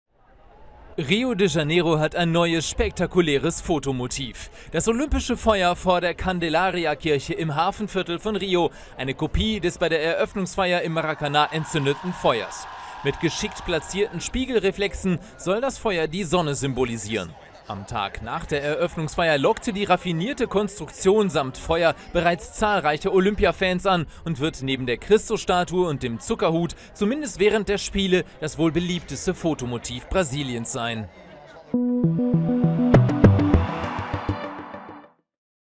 Olympisches Feuer in Rio lockt Brasilianer an
Rio hat ein neues Lieblings-Fotomotiv. Das Olympische Feuer vor der Candelaria-Kirche im Hafenviertel von Rio, eine Kopie des bei der Eröffnungsfeier im Maracana entzündeten Feuers.